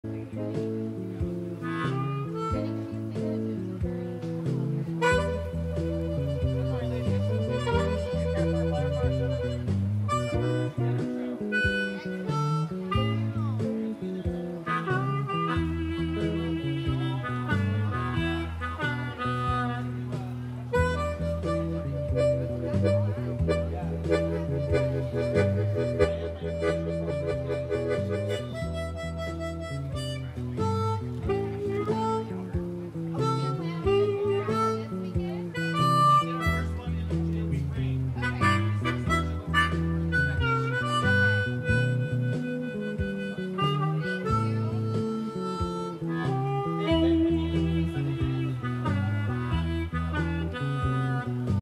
Blues Harmonica Jam at the Modesto Farmers mkt.
Playing Blues through the JBL PRX 1  & Hohner HB52 mic. Seydel harp